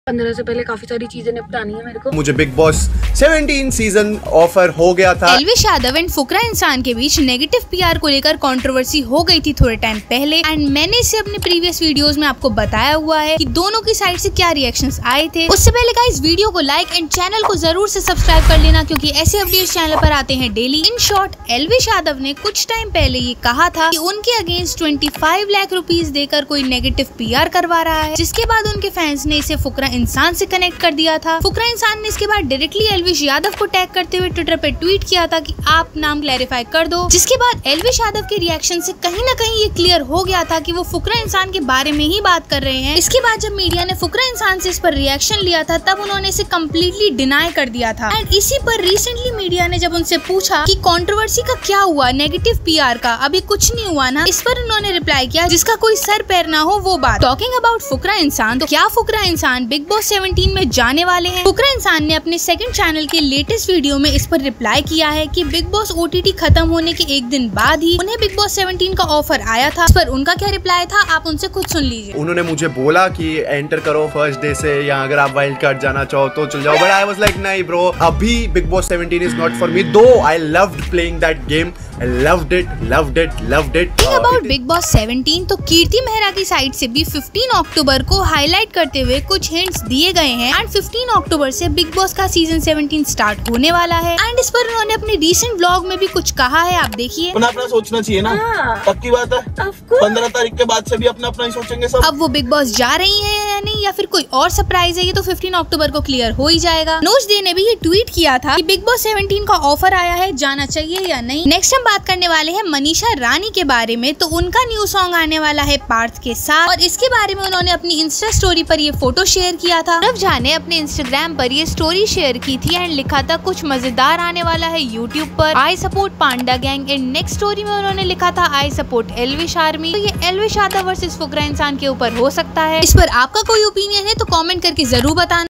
Fukra Insaan ANGRY REPLY to Elvish Yadav? | Fukra Insaan, Kirti Mehra in BIGG BOSS?, Manisha Rani
Kirti Mehra breakup
elvish Yadav breakup